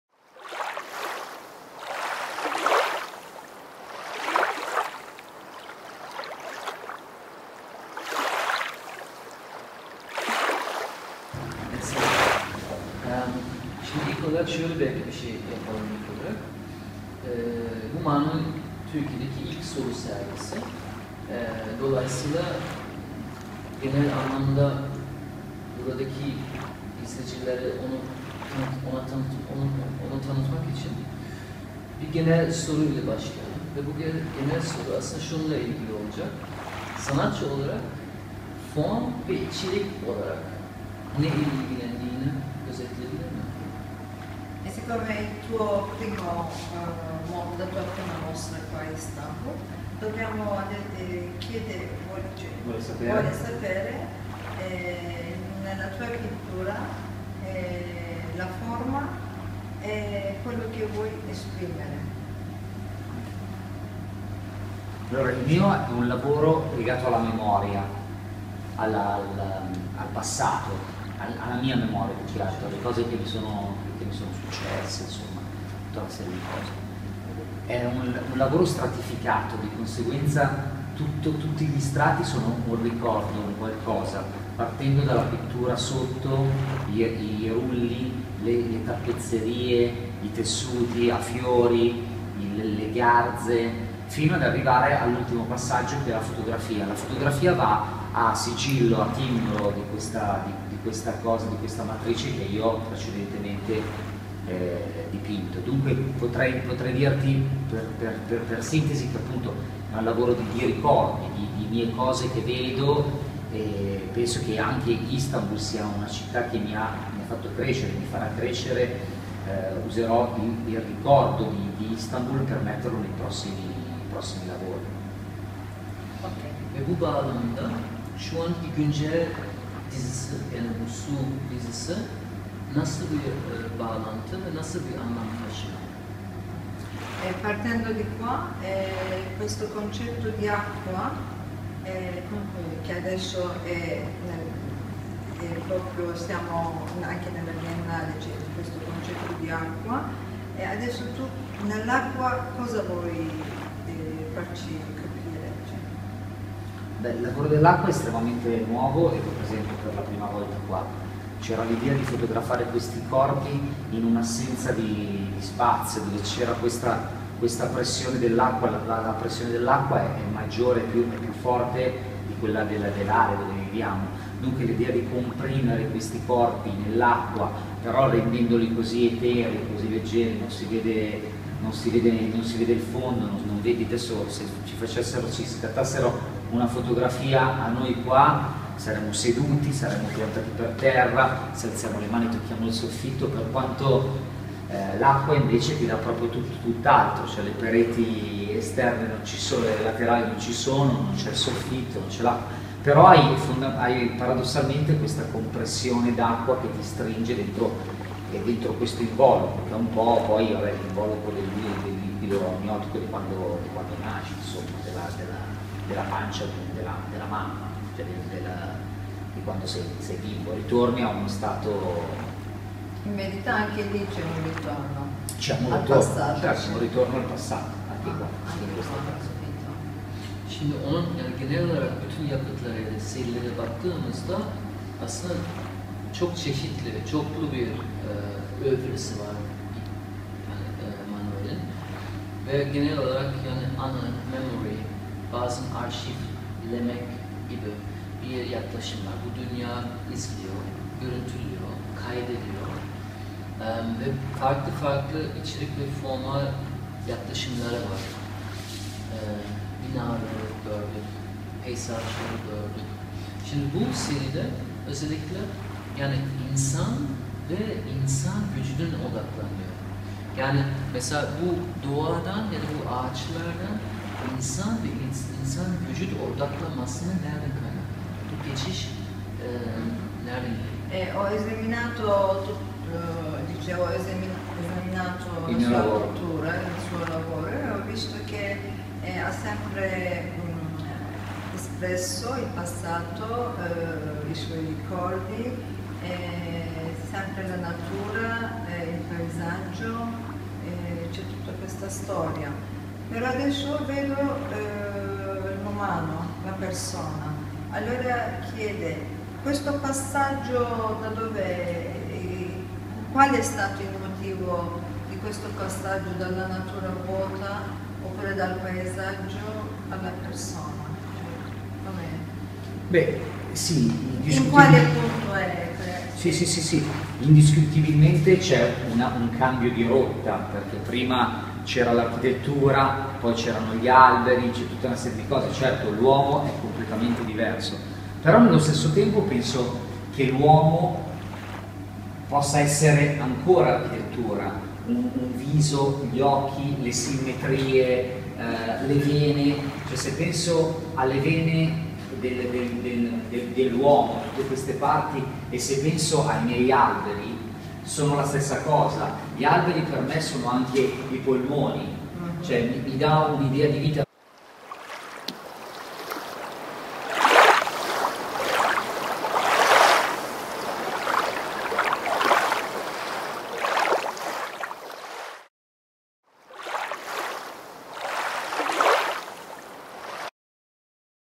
Manuel Felisi Acqua Su
La mostra personale di Manuel Felisi, Acqua,è stata presentata negli spazi della Russo Art Gallery di Istanbul dal 9 settembre al 20 ottobre 2015. La mostra si svolgerà in contemporanea alla XIV Biennale di Istanbul intitolata Saltwater, nella quale la nuova ricerca di Felisi si inserisci perfettamente. Inaugurazione mercoledì 9 settembre ore 18.00.

Boğazkesen Cad. 21/A – Tophane – Istanbul

Orari di apertura: Martedì – Sabato : 11.00 – 18.30

İtalya’nın en köklü galerilerinden Galleria Russo’nun İstanbul’daki mekanı Russo Art Gallery Istanbul, uluslarası İtalyan sanatçı Manuel Felisi’nin “Su” adlı sergisini 09 Eylül 2015, Çarşamba günü sanatseverlere sunuyor.

İstanbul, Paris, Roma, Londra, Milano, Roma, Miami vs. gibi pek çok şehirde kişisel sergisi olan, Contemporary Istanbul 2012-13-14 dahil olmak üzere pek çok şehirde önemli sanat fuarlarına katılmış olan Manuel Felisi; Su ‘Acqua’ adını verdiği yeni kişisel sergisini Russo Art Gallery İstanbul’da sevenlerin ilgisine sunuyor. Ayrıca Felisi’nin eserleri Contemporary İstanbul 2015’te Russo Gallery ile sanatseverler ile buluşacak.

Manuel Felisi’nin 2014 Ekim ayında Russo Art Gallery Roma’da sergilemiş olduğu kişisel sergisi, farklı teknikleri birleştirerek ahenk içinde oluşturduğu kompozisyonlar ile sergiyi gezenlerin büyülenmelerine sebep oldu, Roma koleksiyonerleri, sanat eleştirmenleri ve sanatseverler tarafından beğeni topladı.

Bu sergi için sanatçı Manuel Felisi, kendisini tanınmasının sağlayan resimlerinin tipik ve özlü konularını içeren özel eserler yaratmış. Birçok tekniği bir arada kullanarak şiirsel bir tema ile anlattığı kompozisyonları Russo Art Gallery İstanbul’da 20 Ekim 2015’e kadar ziyaret edilebilir.

Adres: Russo Art Gallery İstabul, Boğazkesen Cad. No: 21/A Tophane- İstanbul